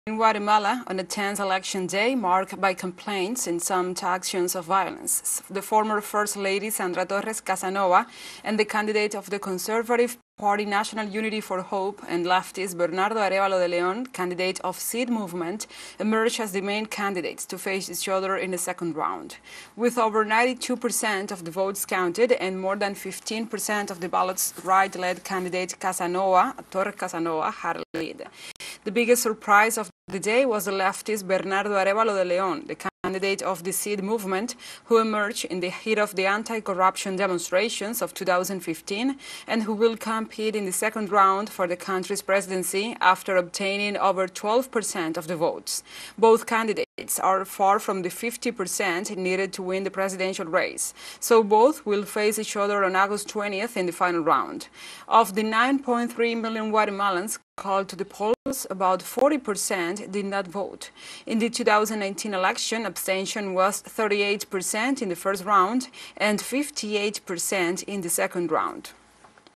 Candidates Torres and Arevalo to compete for Guatemala's presidency in runoff election
Sandra Torres Casanova, candidate for the Presidency for the National Unity of Hope (UNE), and Bernardo Arévalo de León, candidate for the first magistracy for the Seed Movement, will dispute the Presidency of Guatemala in a runoff, in an election where the null vote and abstention stood out. teleSUR